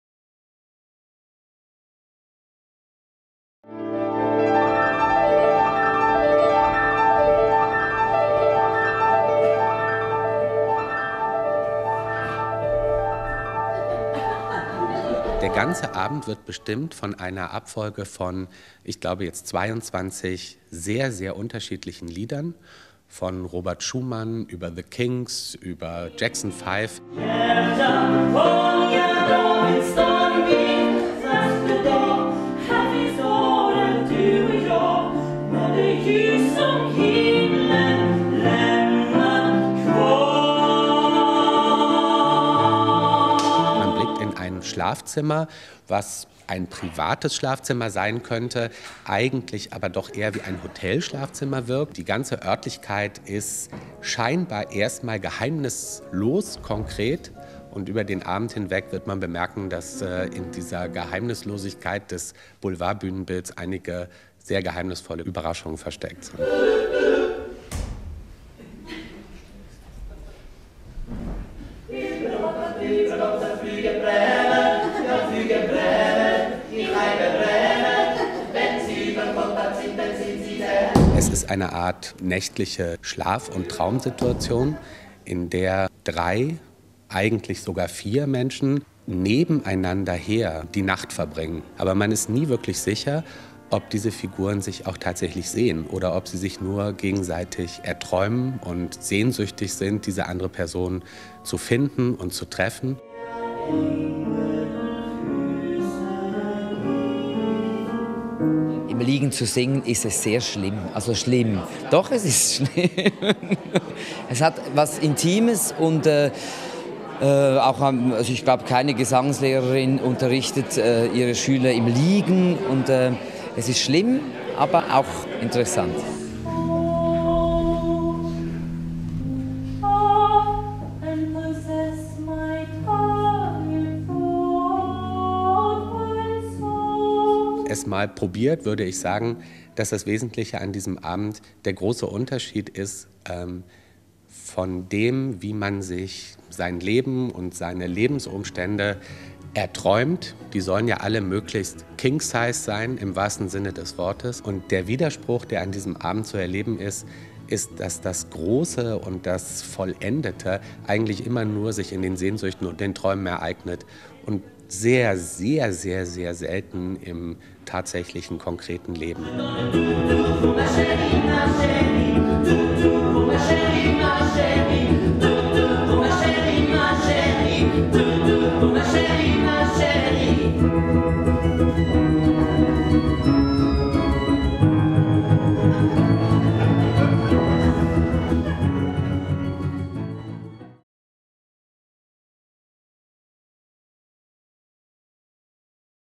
King Size - DNS 02. & 03. juni - Tora Augestad - Christoph Marthaler
Videoproduksjon: art-tv.ch 2013

Absurd komikk og bittersøt kjærlighet i King Size-sengen.

Hva har Carola, Brahms, The Jackson Five, Schumann, Kinks og Purcell til felles? Like mye som mann og kvinne, drøm og virkelighet?

King Size er en musikalsk teateraften om kjærlighet, drømmer, umulig kommunikasjon og forvekslinger. Bergenseren Tora Augestad har gjort internasjonal suksess med denne vanvittige forestillingen signert den sveitsiske regissøren Christoph Marthaler. Og det er ingen tvil om hvorfor.

Augestad og medspiller Michael von der Heide briljerer både innen musikk og komikk, og i kjent Marthaler-stil preges det hele av livets absurditeter, lek og musikkens formspråk.  De to spiller (kanskje) et par som har tatt inn på et hotell. De prøver å sove, men sengen og drømmene er for store, og de blir stadig forstyrret av seg selv og mystiske skikkelser.

Marthaler har vært en av de mest markante skikkelsene i europeisk musikkteater de siste 30 årene. Hans surrealistiske og melankolske forestillinger viser mennesker som prøver å kommunisere, men stort sett mislykkes.

Augestad er utdannet kabaretsanger og arbeider nå i hele Europa. I 2012 debuterte hun på operaen i Zurich, og siden har hun hatt flere roller i Marthalers forestillinger og opptrådt på ulike konsertscener med alt fra barokk til kabaret, moderne musikk og jazz.